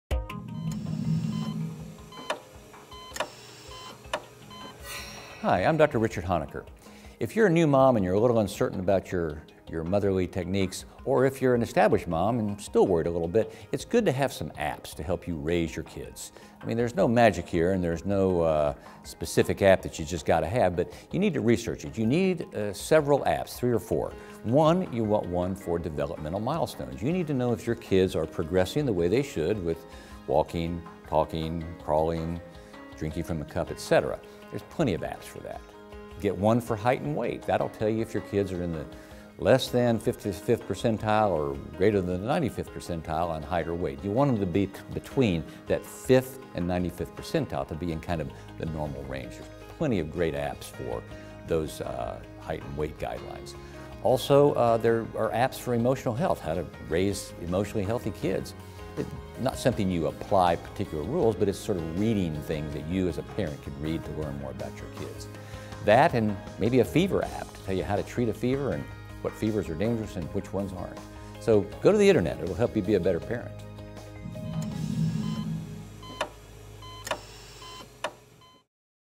Mobile Apps for Moms
Dr. Honaker discusses a few ways in which technology and your phone can assist you in taking care of your child and help make it easier for you to raise them.